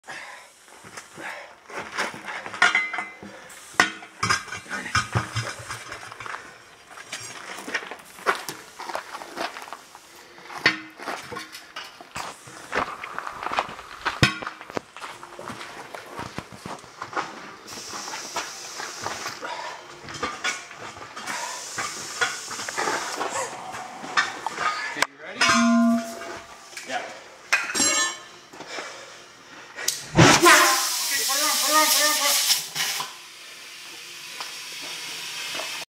Tire set on bead with Starting Fluid
Here is how we set tires on the bead without a tire blaster-NOTE THIS ONLY WORKS IF YOU ALSO HAVE A WAY TO INFLATE THE TIRE. This is a 11R 24.5 Truck tire. This is how we do it in the woods if we bend a rim as well. Just use a sledge hammer to straigten the rim, and then dig a hole under the wheel so it is just hanging-not touching the ground, and then use the ether trick to set the bead. as soon as the ether explodes, you MUST get air to the tire, or this will not work; it will just deflate again.